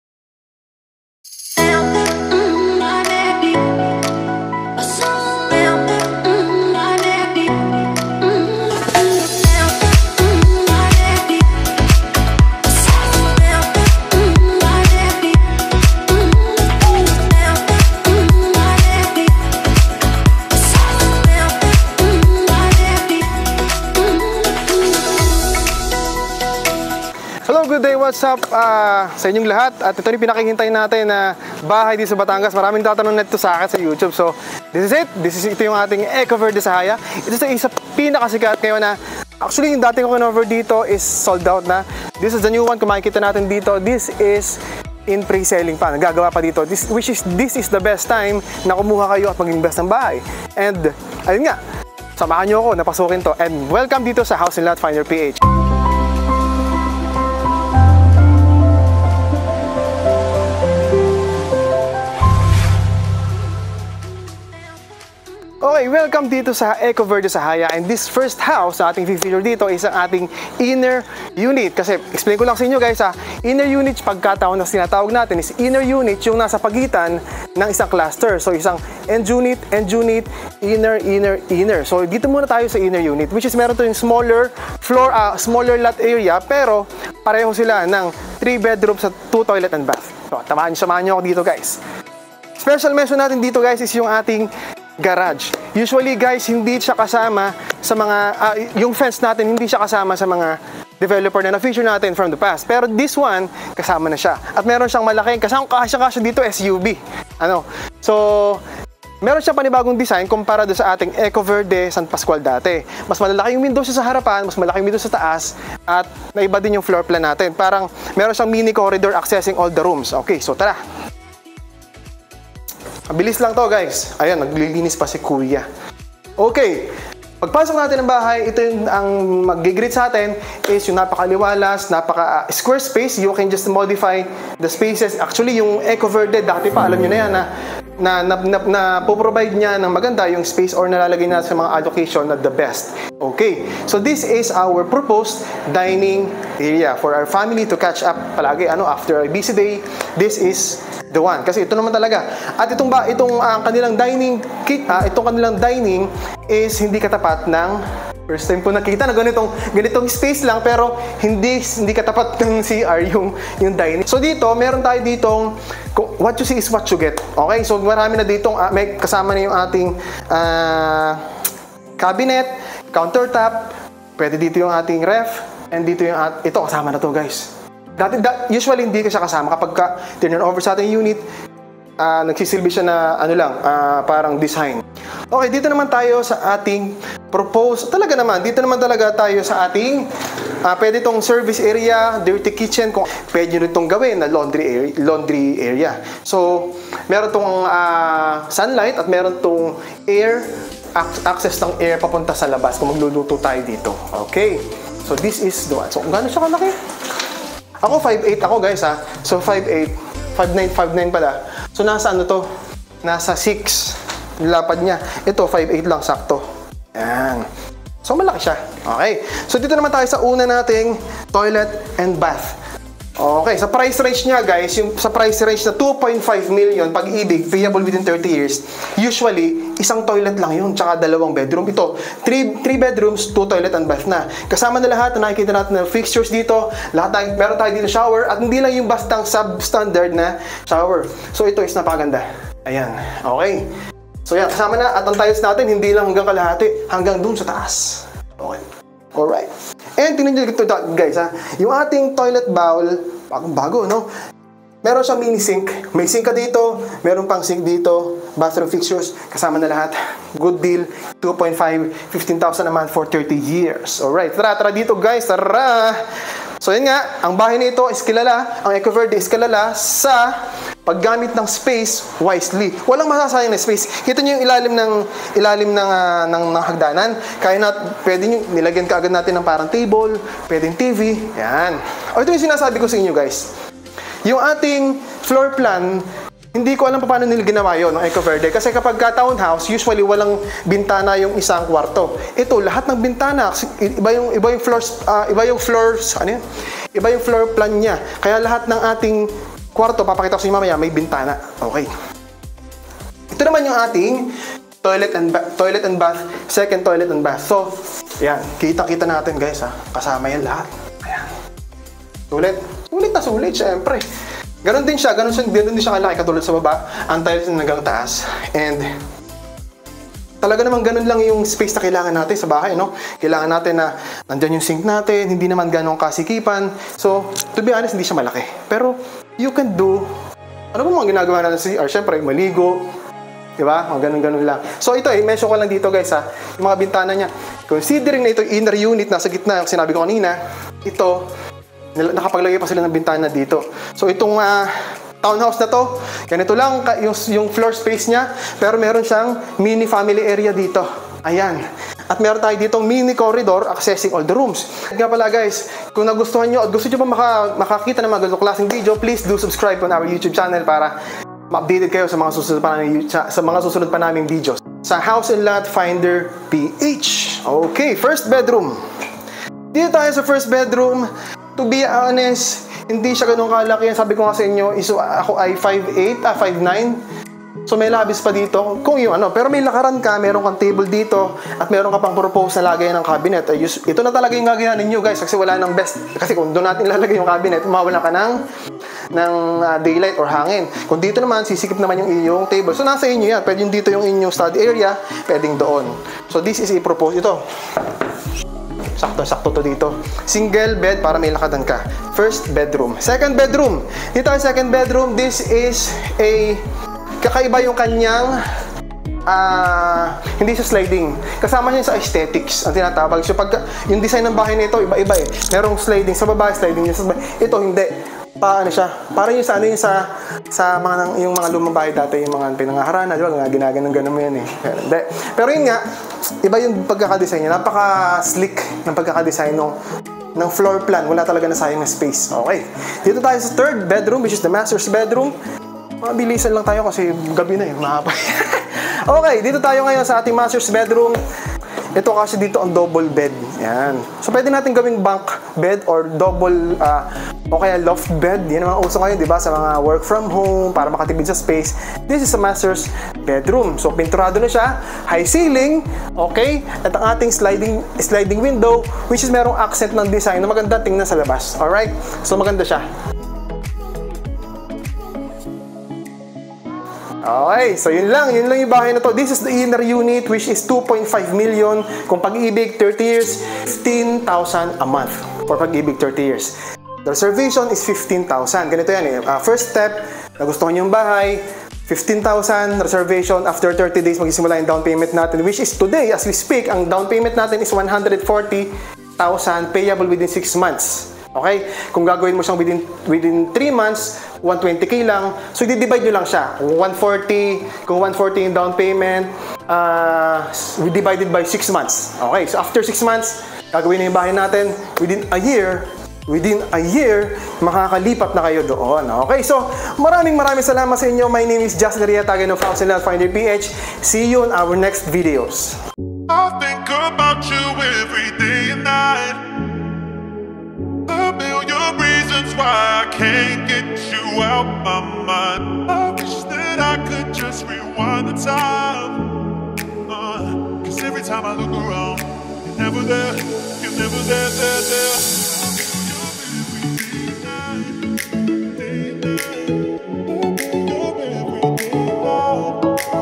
They SOLD 45 houses in 1 week ...
Bahay na 3 bedrooms 2 Toilets, may Garage FENCE PA ?
Reserve yours now for only 14k!
𝐄𝐜𝐨𝐯𝐞𝐫𝐝𝐞 𝐇𝐨𝐦𝐞𝐬 𝐒𝐚𝐡𝐚𝐢𝐚. BATANGAS
Located @ Batangas City ! Near Bypass road !
Lot Area of 58sqm
Floor Area of 63sqm

Viber/Whatsapp
Txt/Call: